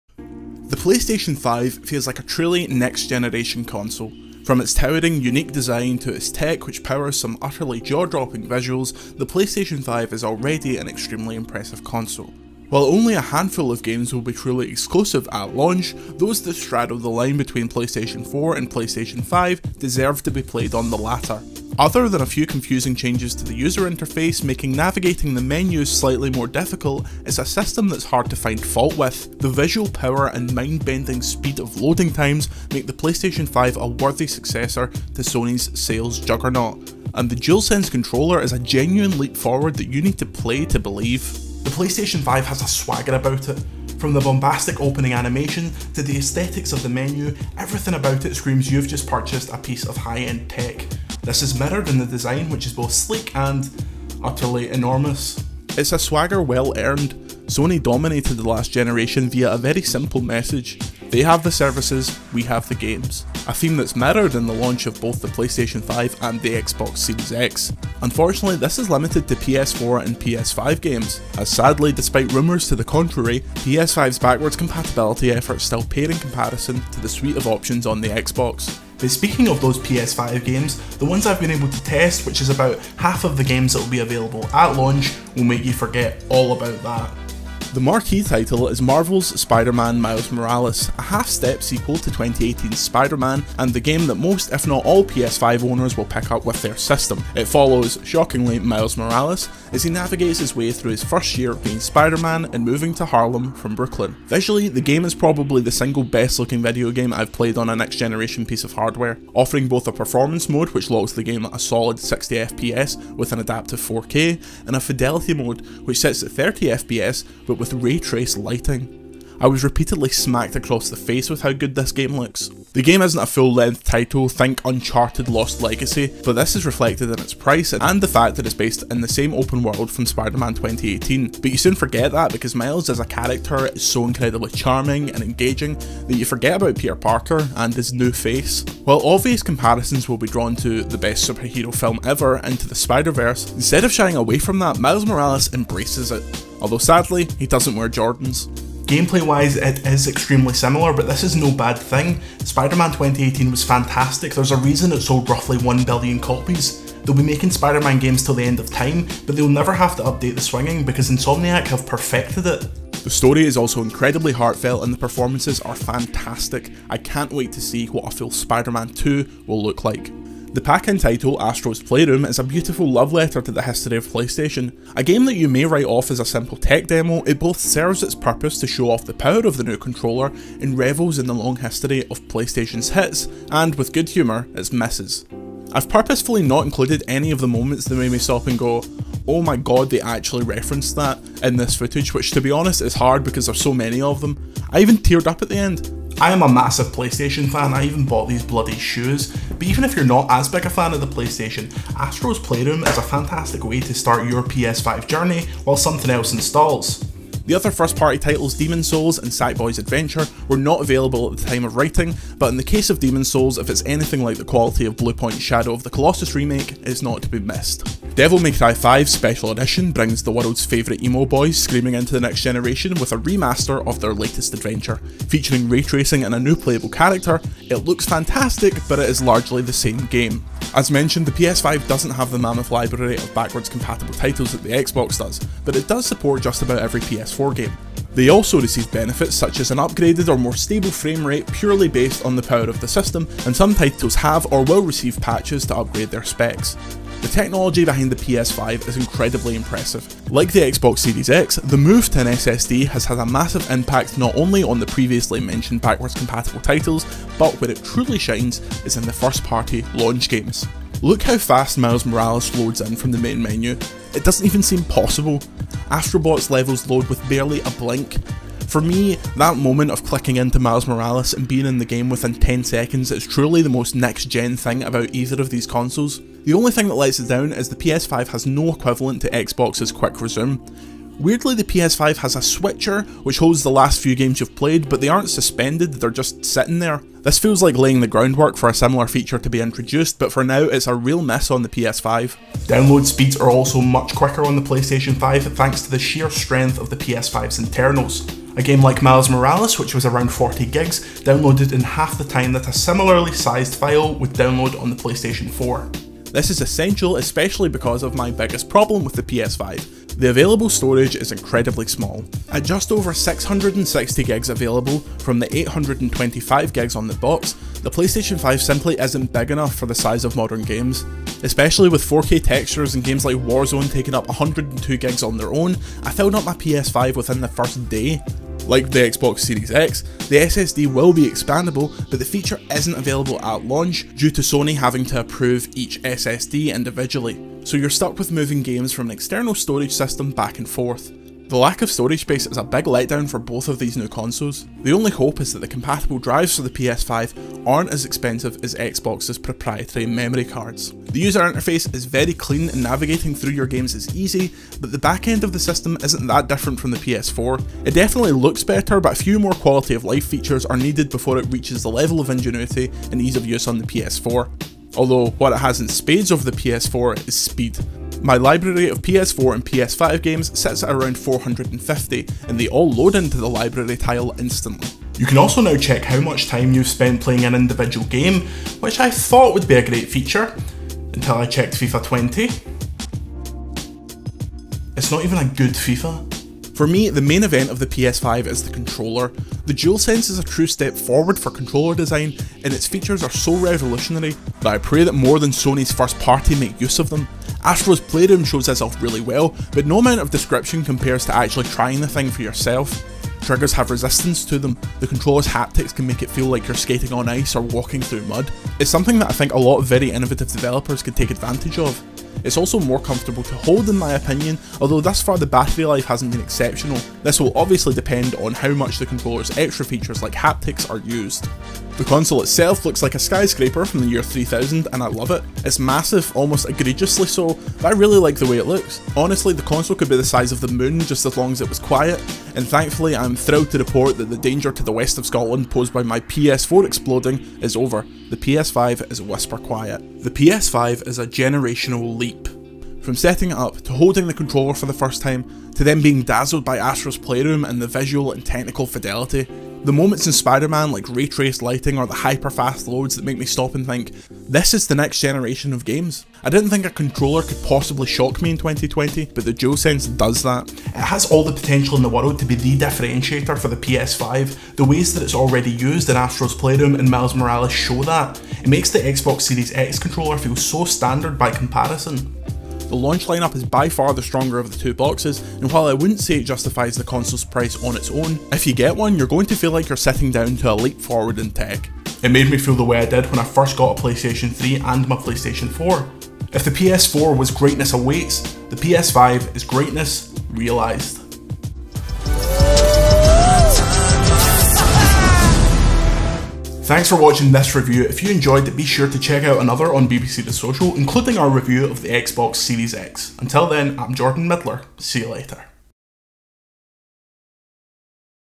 Our Playstation 5 review! - BBC The Social
More from The Social at bbc.co.uk/thesocial

The PlayStation 5 feels like a truly next-generation console. From its towering, unique design to its tech which powers some utterly jaw-dropping visuals, the PlayStation 5 is already extremely impressive.

While only a handful of games will be truly exclusive at launch, those that straddle the line between PS4 and PS5 deserve to be played on the latter. Other than a few confusing changes to the user interface making navigating the menus slightly more difficult, it’s a system that’s hard to find fault with.

The visual power and the mind-bending speed of load times make the PS5 a worthy successor to Sony’s sales juggernaut and the DualSense controller is a genuine leap forward that you need to play to believe.

Reviewed by Jordan Middler

The Social | BBC